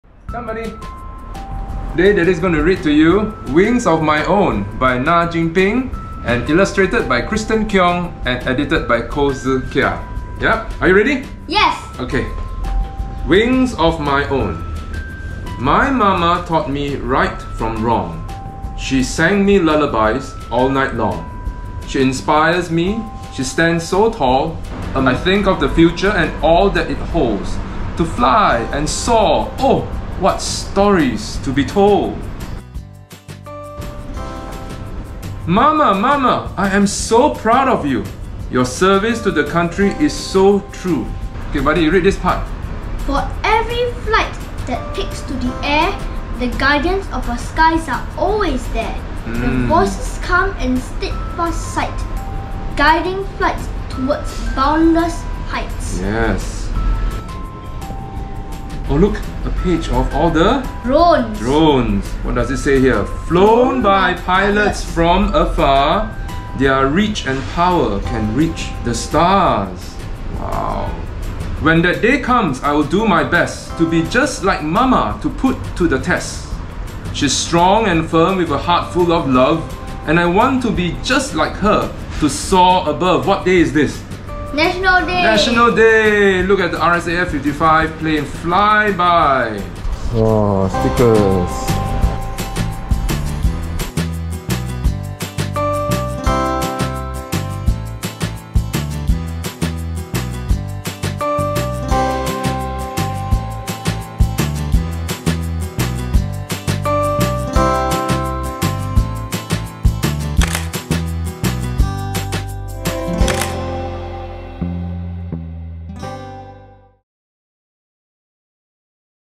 Daddy Reads - Wings of My Own
Remember how you both 'fought' over reading this precious book by Maj Nah  'Wings of My Own'?  Actually Daddy and Mommy also wanted to read it badly too but we gave you guys a chance first!
What a precious limited edition book where we got to hear from our friend Maj Nah her as she shares her perspective and experience as a mother and an RSAF pilot!
This is such an interesting book with beautiful illustrations, an inspiring story plus some fun within too! A collectors' item indeed.

Here's how to get this limited edition book: Register and attend a reading session of it. Hurry last few slots left :

With @TheRSAF